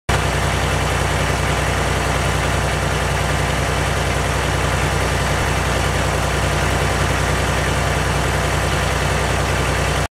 GovDeals: 1995 Ford LTS9000 Tandem Axle Roll Off Truck (10.5
1995 Ford LTS9000 Tandem Axle Roll Off Truck (10.5L L6 Diesel) sold by State of South Carolina on GovDeals.com